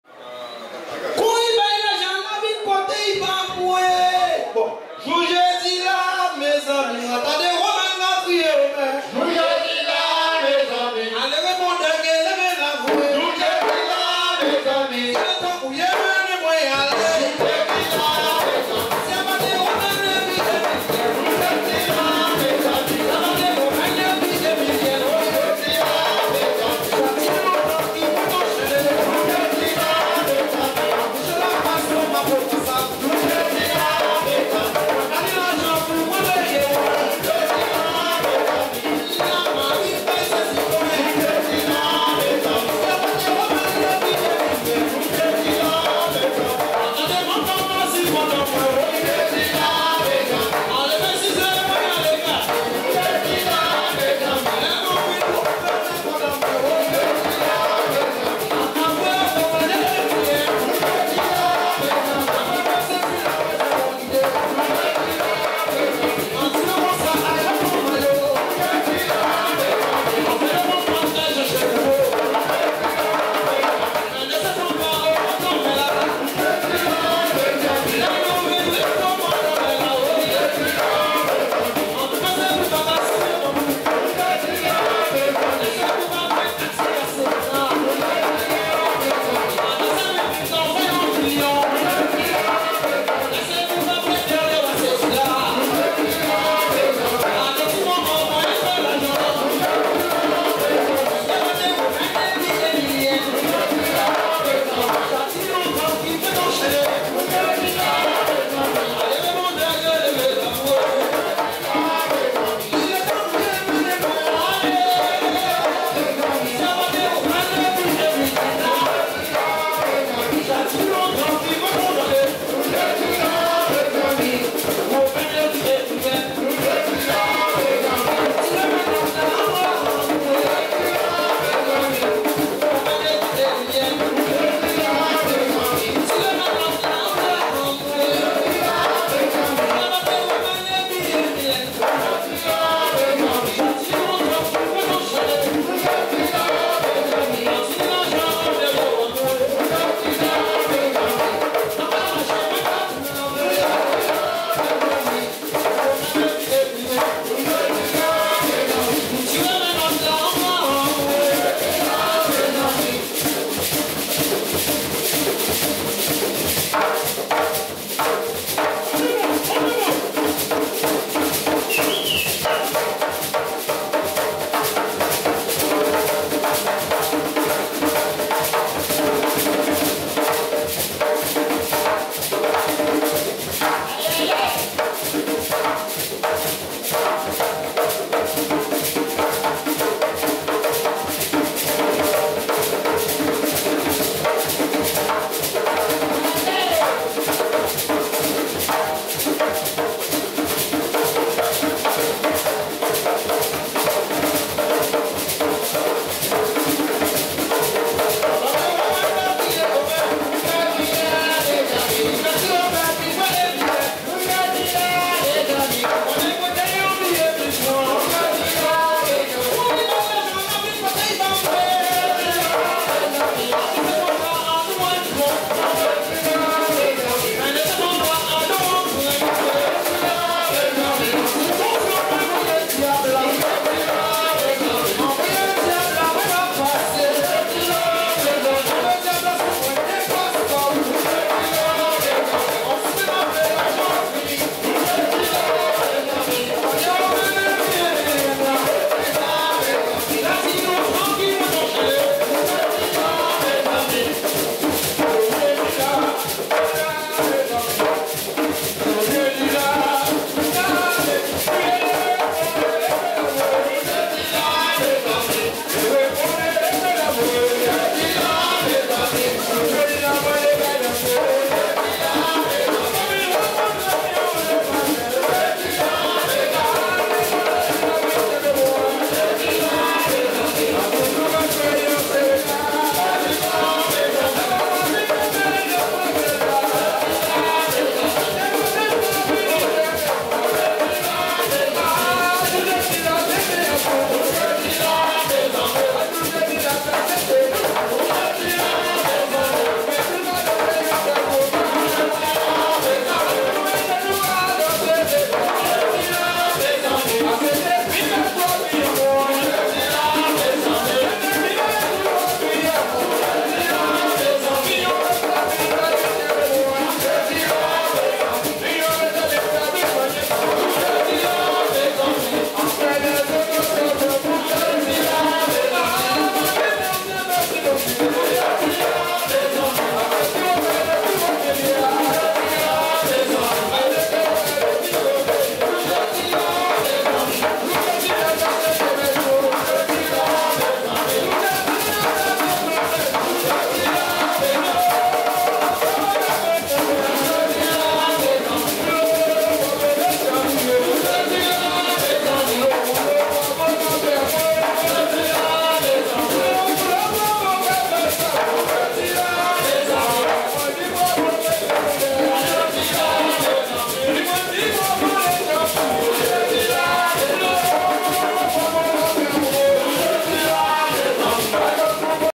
Guadeloupe: La Baie des Pelicans
Live performances in Guadeloupe you can find just mindlessly wandering around the island... 😂